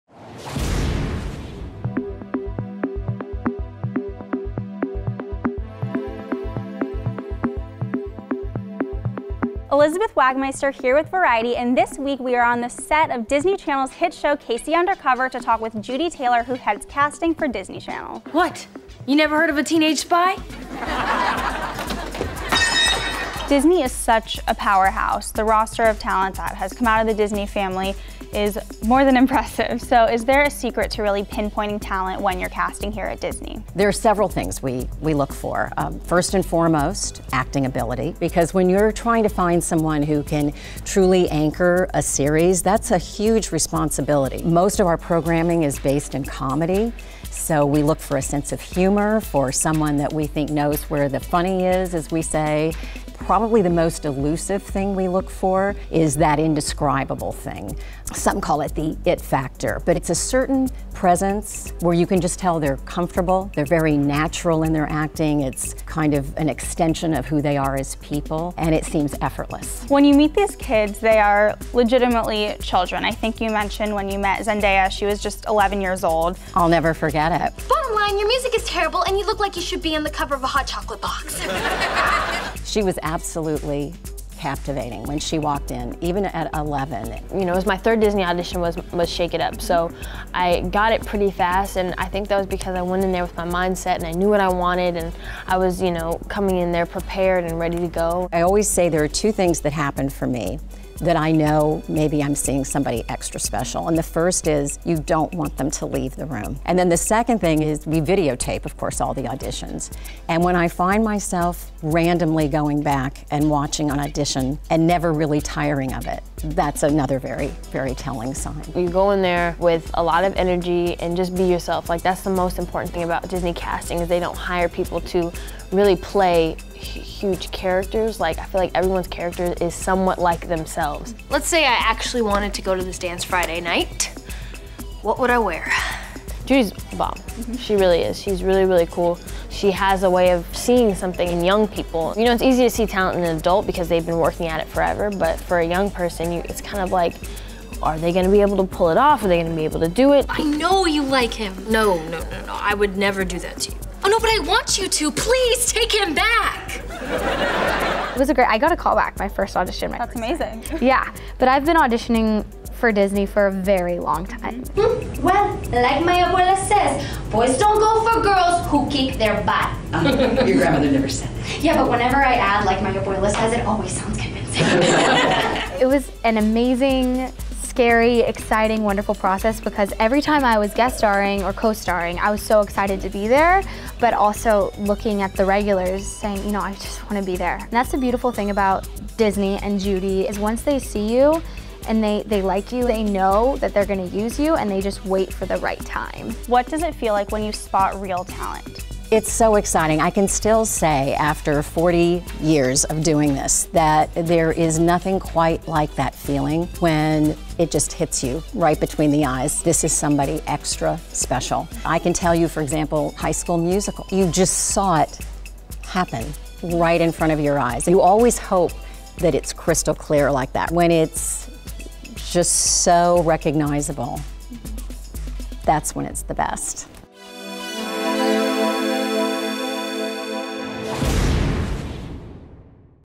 Variety Artisans: Spotting Stars - Casting Kids for Disney Channel
Disney Channel has an uncanny record for turning up stars: Shia LaBeouf, Miley Cyrus, Selena Gomez, Zac Efron, Vanessa Hudgens and more. Elizabeth Wagmeister goes behind the scenes with Disney Channel’s chief of casting, Judy Taylor, and with “K.C. Undercover” stars Zendaya and Veronica Dunne, to find out how they do it.

Writer & Producer: David S. Cohen
Host: Elizabeth Wagmeister
Supervising Producer: Preston Northrop
Camera: Preston Northrop, Sal Castellanos
Editor: Armando Aparicio